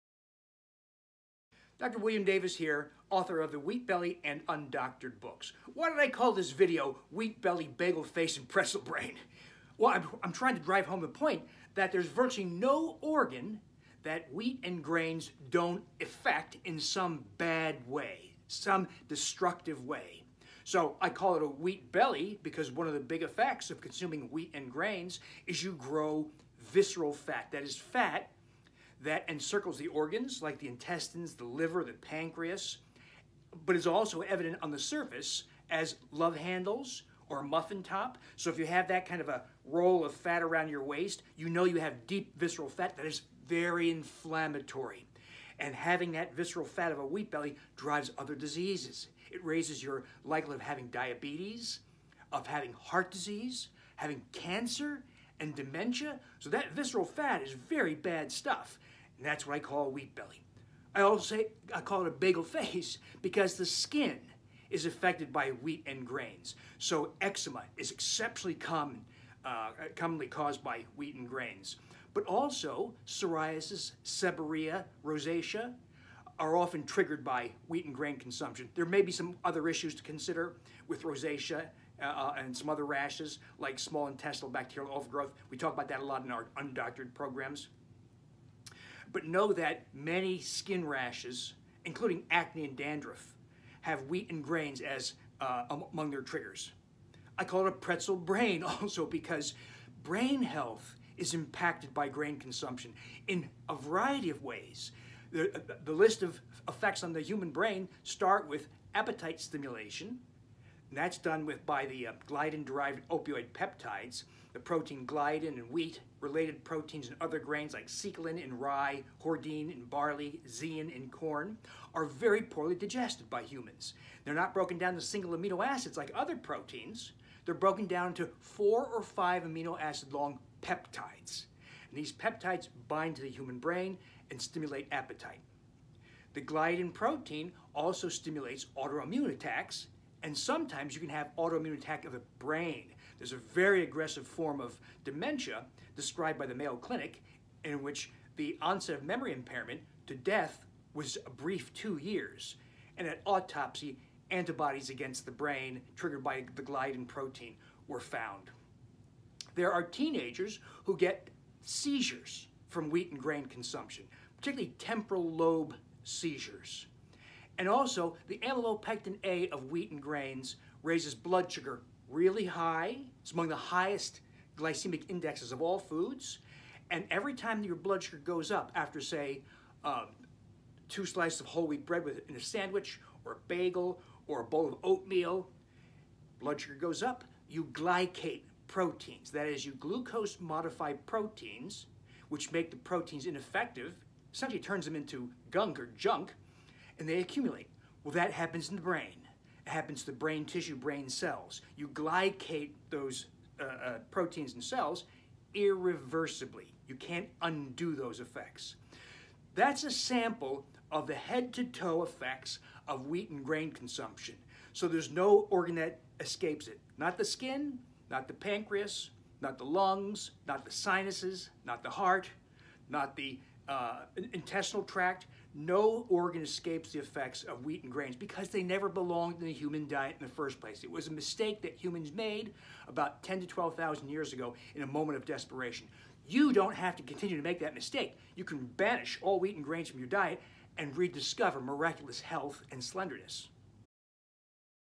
Wheat Belly, Bagel Face, and Pretzel Brain
There's a lot more to the destructive health effects of wheat and grains than gluten. And there is no organ system in the body that is not adversely affected by wheat/grain consumption.

And find Wheat Belly and Undoctored conversations on Facebook, Twitter, Instagram.